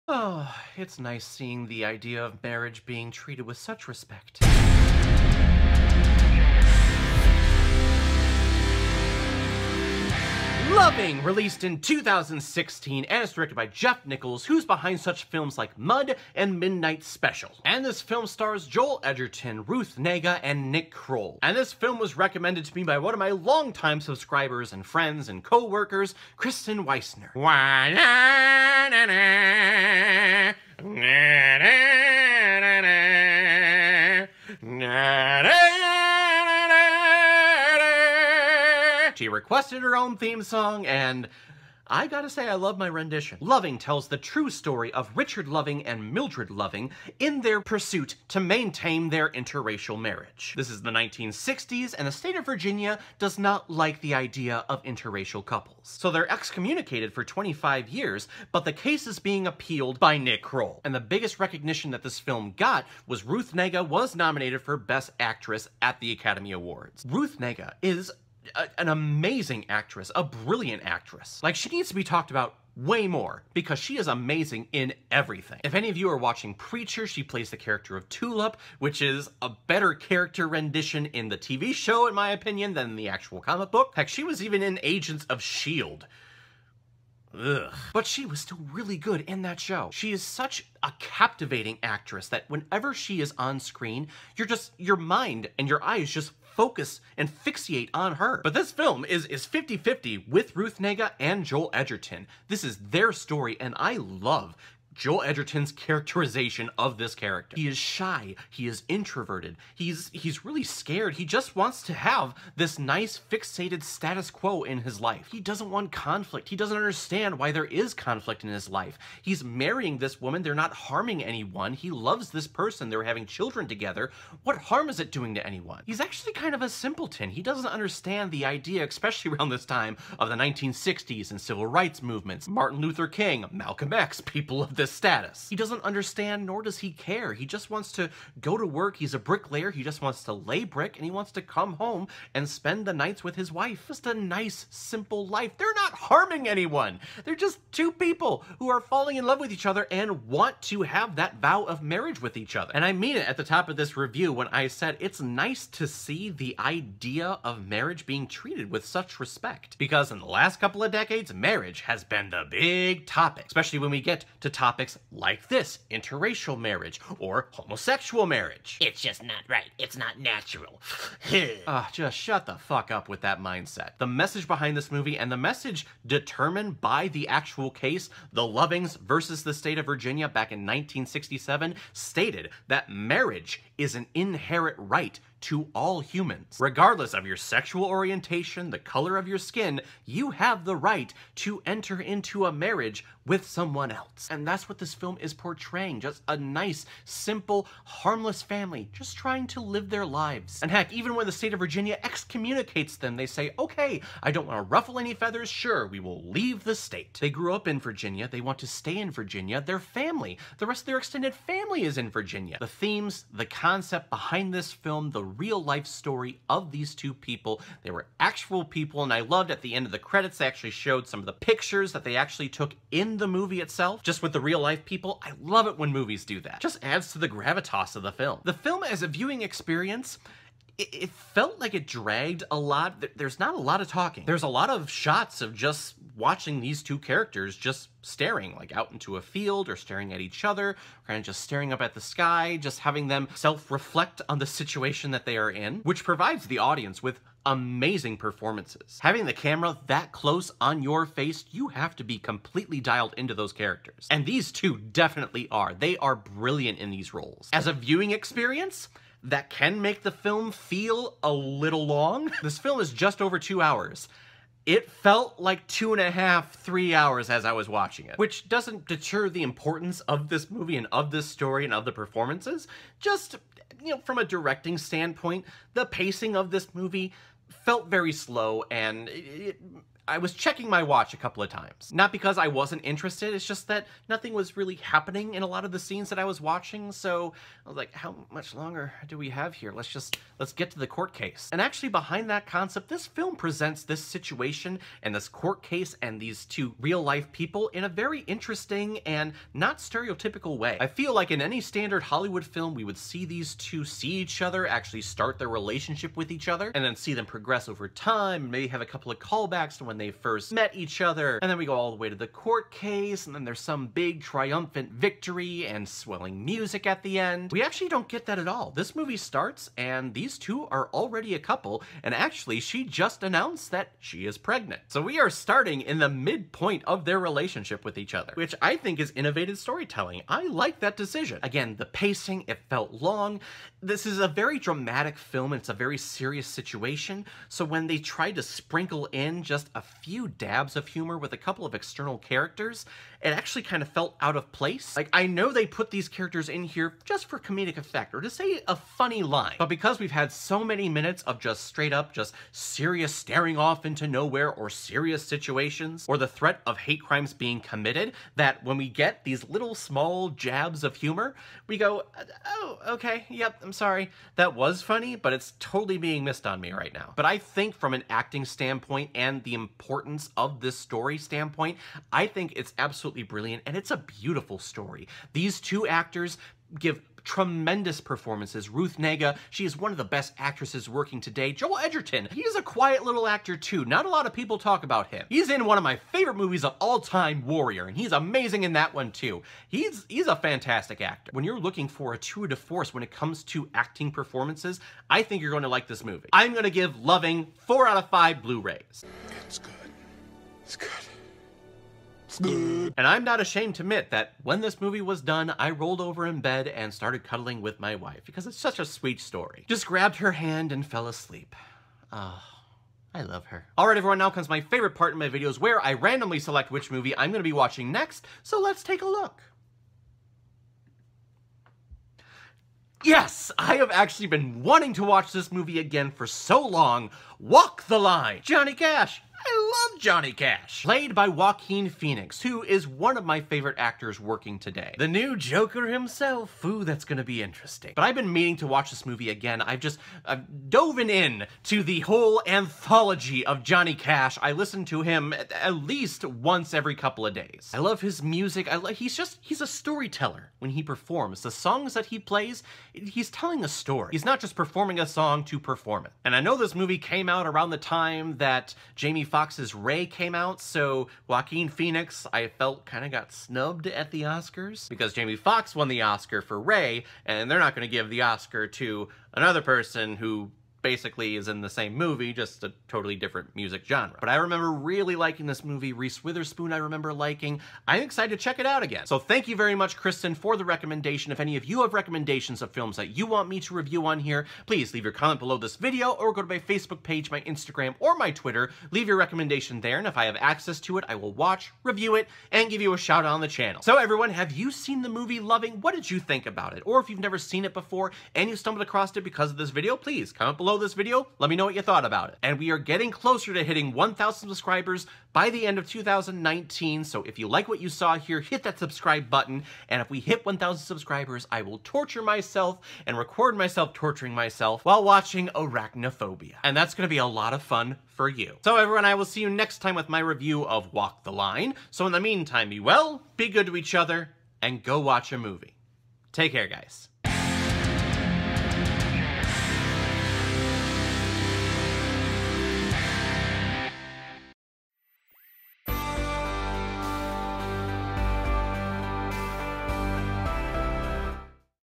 LOVING MOVIE REVIEW
Loving tells the true story of Richard and Mildred Loving fighting for their right to marry each other, even though one of them is white and the other is black.

Please post you recommendations for movie reviews in the comments